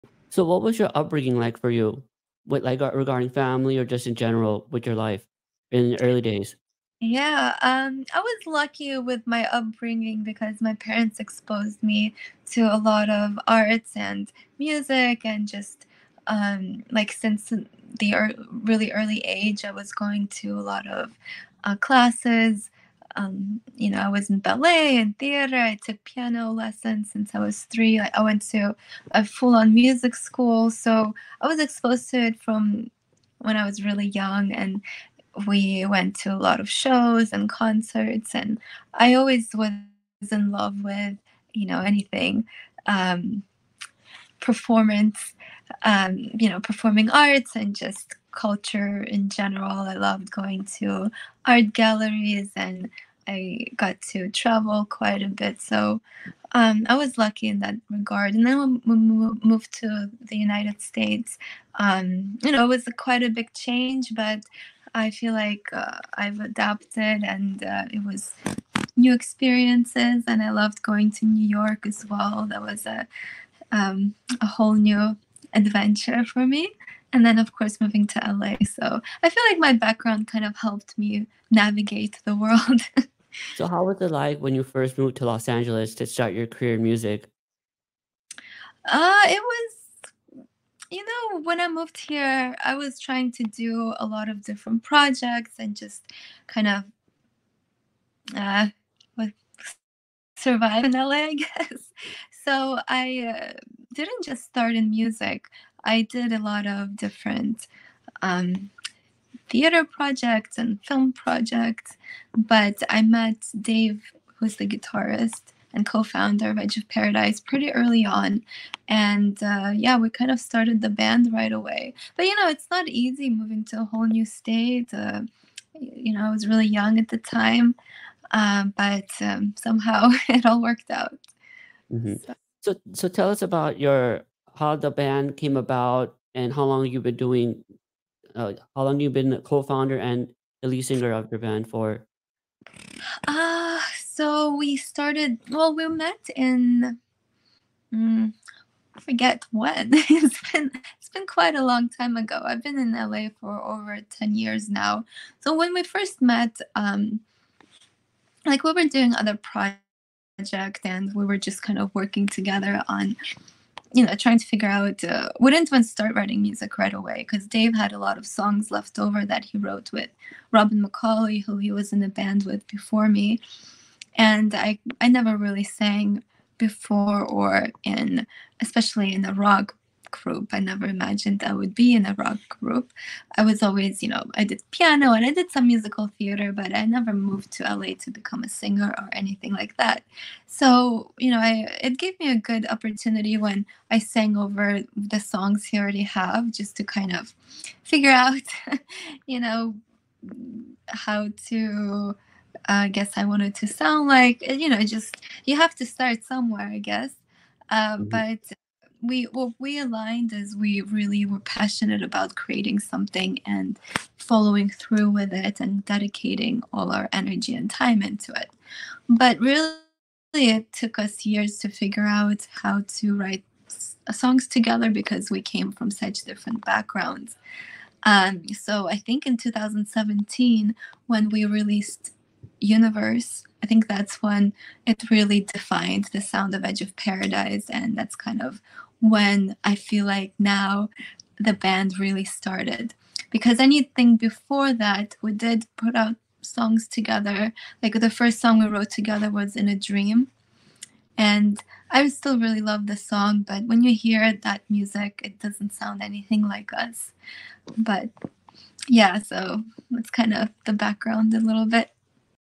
Short Clip of Interview with Margarita Monet
Lucky upbringing with arts and music. From ballet to piano, I embraced culture. Moving to LA was a new adventure.